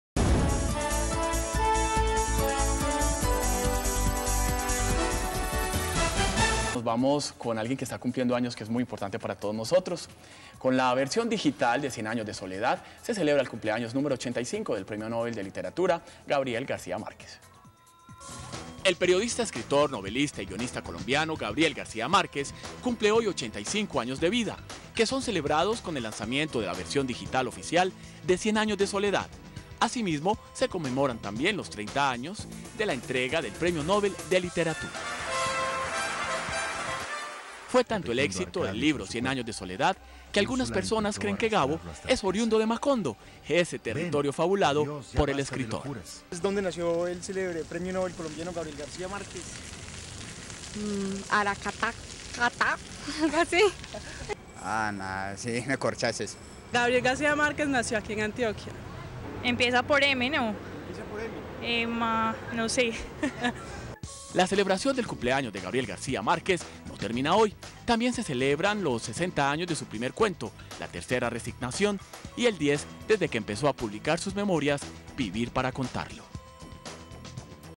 El Nobel Gabriel García Márquez cumple hoy 85 años
Con la versión digital de Cien Años de Soledad se celebra el cumpleaños número 85 del premio Nobel de Literatura Gabriel García Márquez.